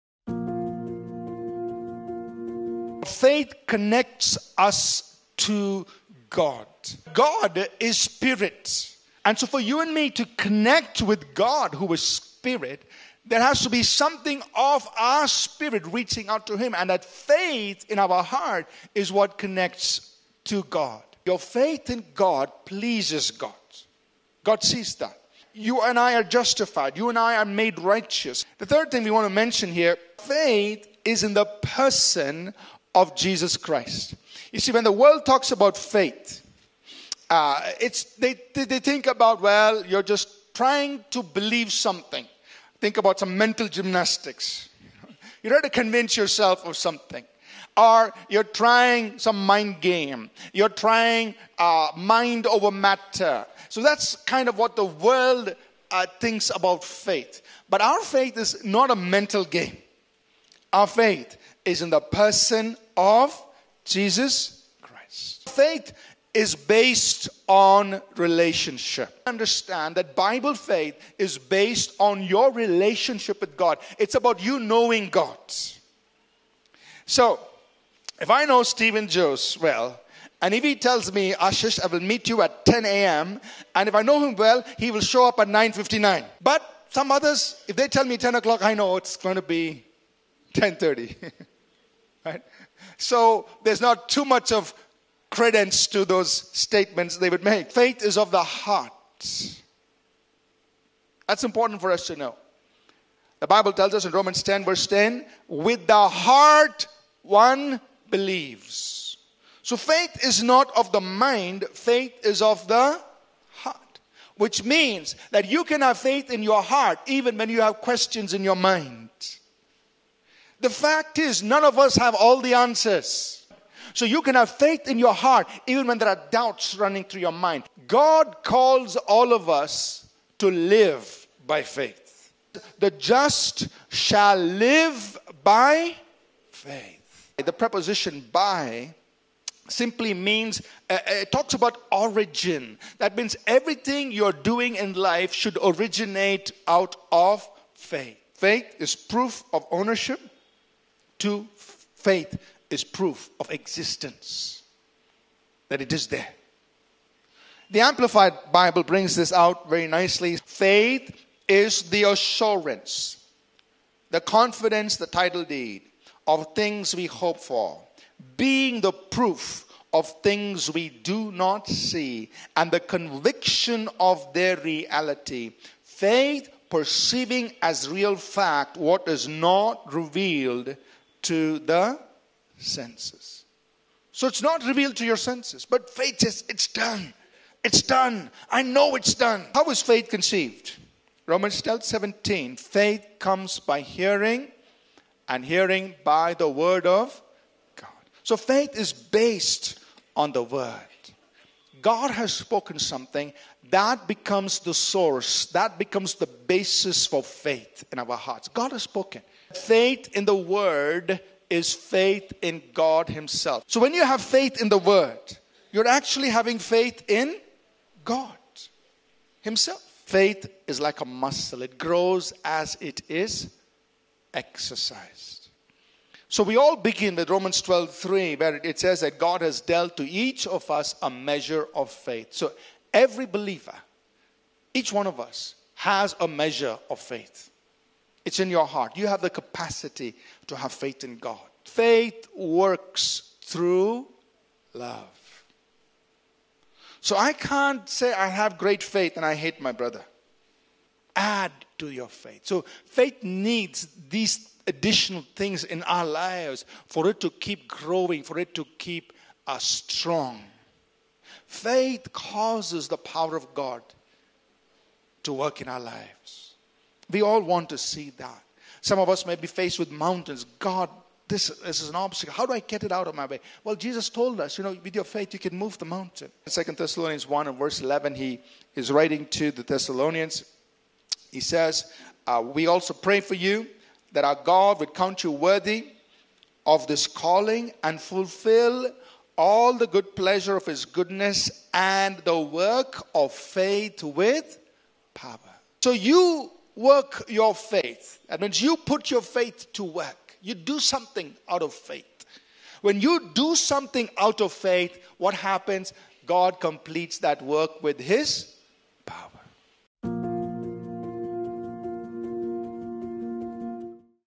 Key Points : Part-1 : Faith : An Introduction
In this introductory message we cover important and fundamental insights concerning faith.